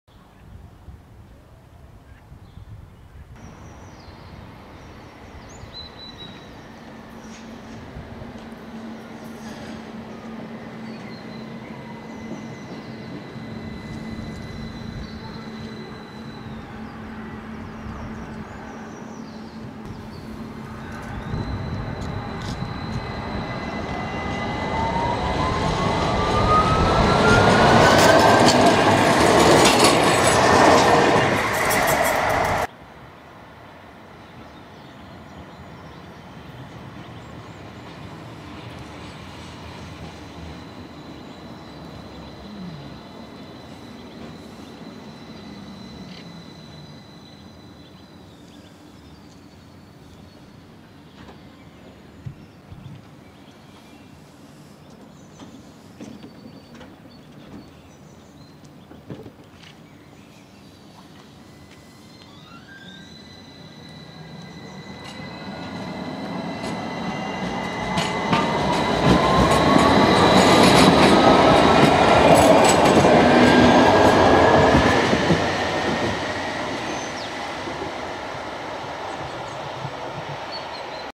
SL Tram Nockebybanan, Höglandstorget, Stockholm
May 2013
Nockebybanan is a tram line between Nockeby and Alvik in the western suburbs off Stockholm, Sweden. The line is 5.6 kilometers (3.5 mi) long, all double track.
Trams are Class A32, type Bombardier Flexity Swift.
6-axled articulated double-ended partially low-floor motor car.